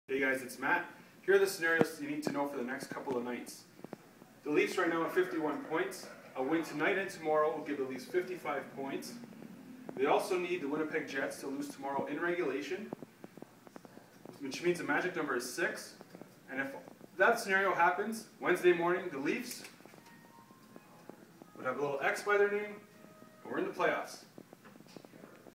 Leafs Playoff Scenarios
How the Leafs can clinch a playoff berth over the next two games.